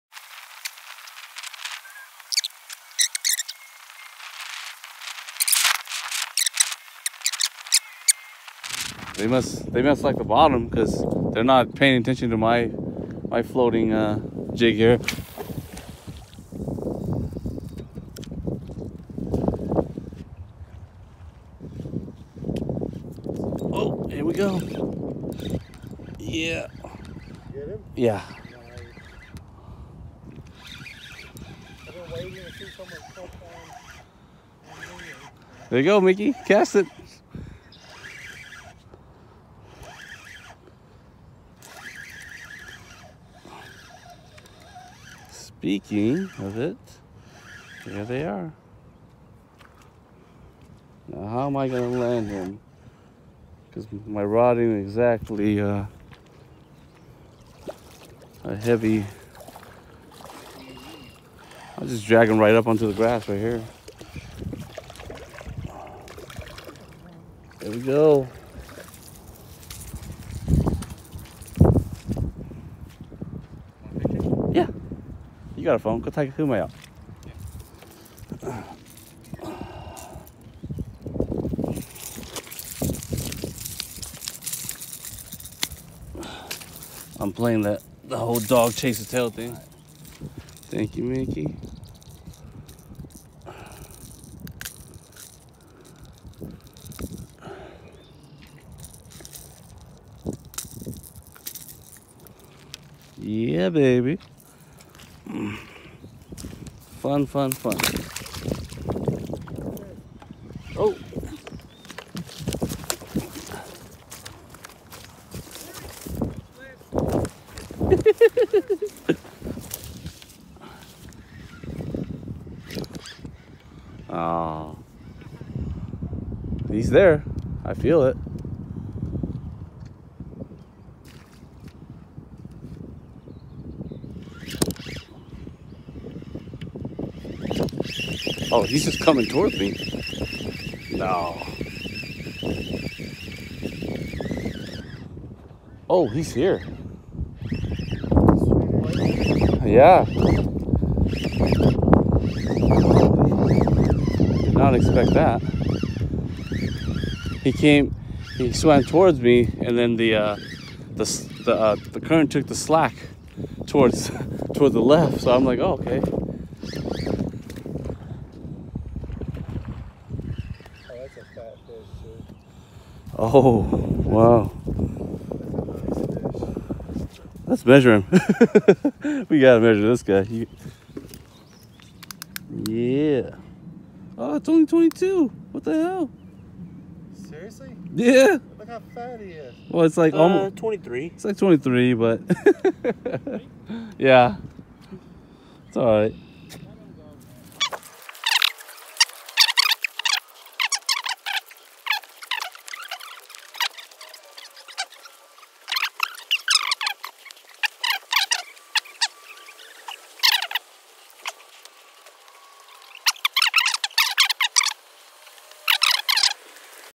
Striper action back east with my famz
Another one of my favorite spots back east where I used to catch stripers regularly.